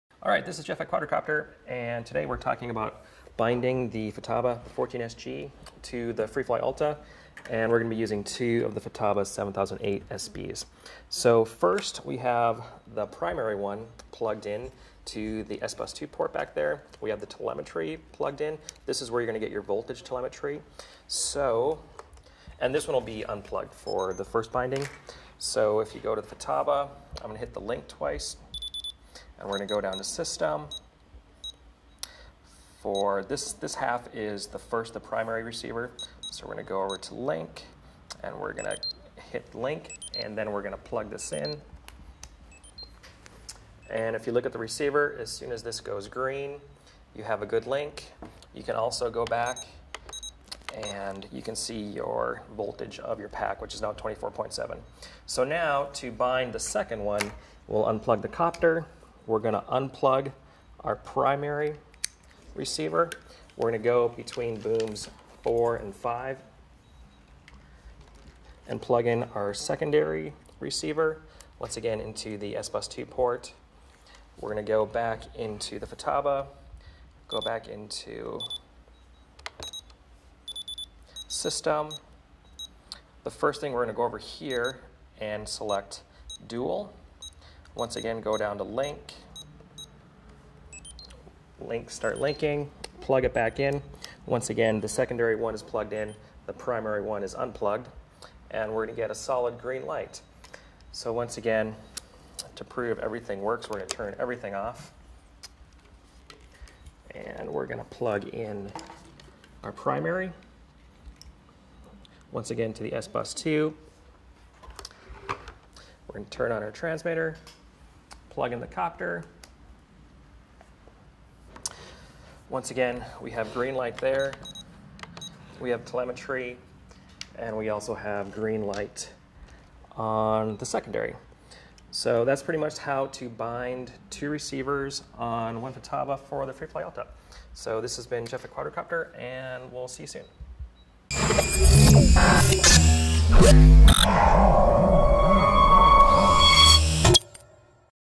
Quadrocopter Tutorial - Linking the Futaba 14SG to the Freefly ALTA
In the tutorial video we show how to link 2 7008SB receivers to a Futaba 14SG transmitter on the Freefly Systems ALTA